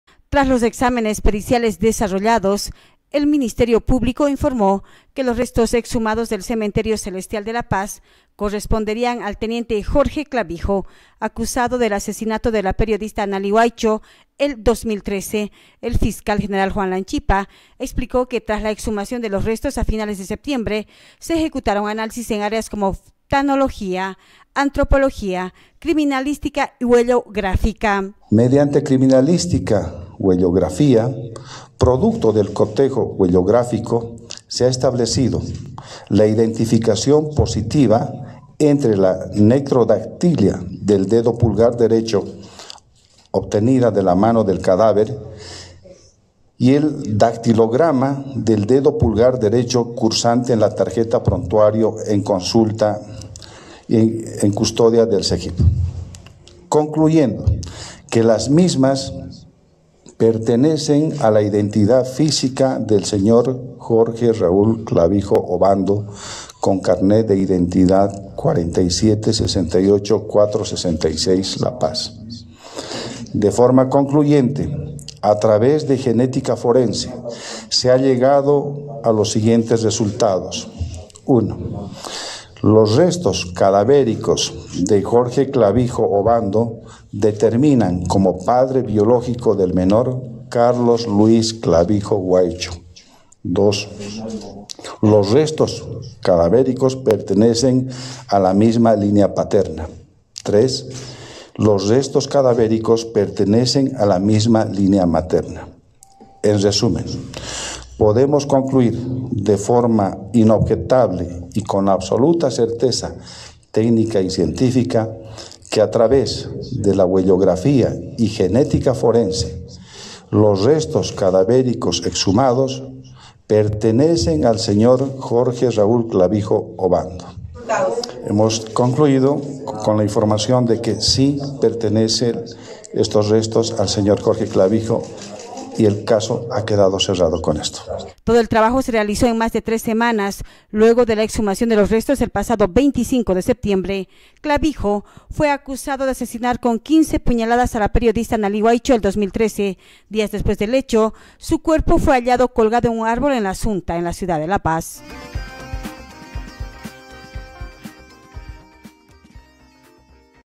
CONFIRMAN QUE RESTOS EXHUMADOS PERTENECEN A JORGE CLAVIJO
Tras los exámenes periciales efectuados, el Ministerio Público informó que los restos exhumados corresponden al Tnte. Jorge Clavijo, acusado del asesinato de la periodista Analy Huaycho el año 2013.